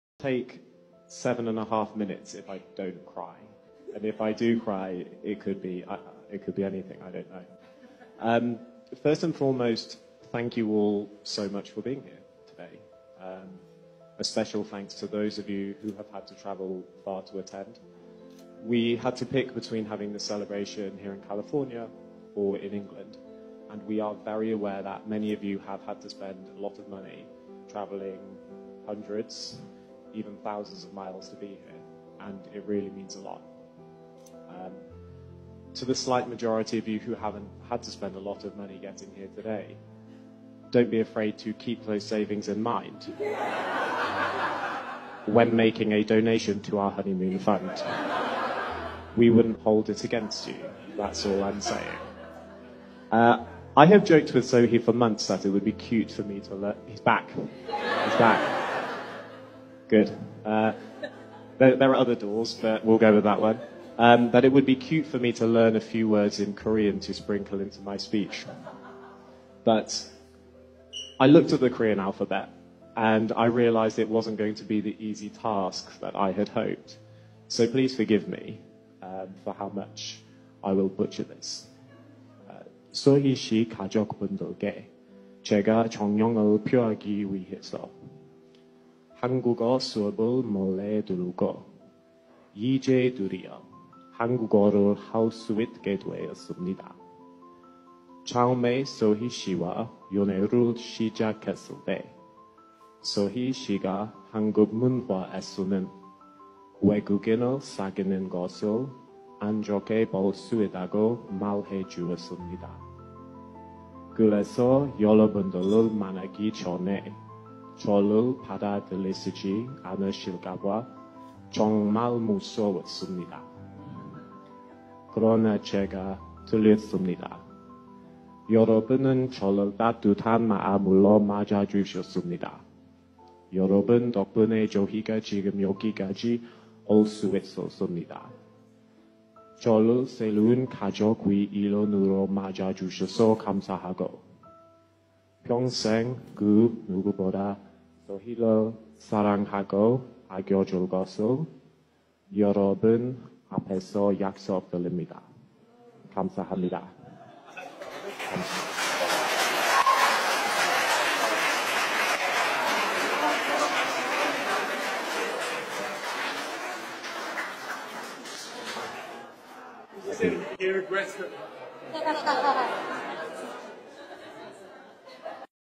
Groom secretly learns Korean, to suprise his wife.
This Groom reveals he was secretly sneaking off for 30 minute video calls for nearly a year to learn Korean and surprise his wife and her family, that is love!

Credits video By: @imjustbait on tiktok